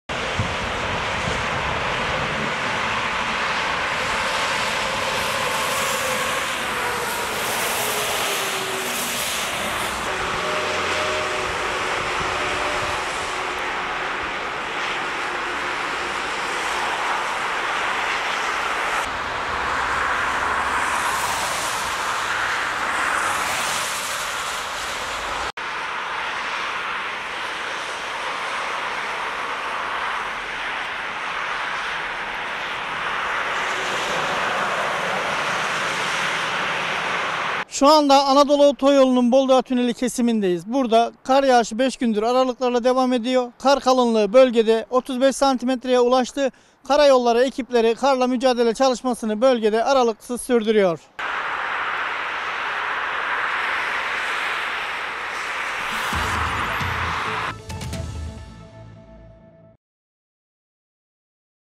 Bolu Dağı'nda kar kalınlığı 35 santimetreye ulaştı
Anadolu Otoyolu'nun Bolu Dağı kesiminde kar kalınlığı 35 santimetreye ulaştı.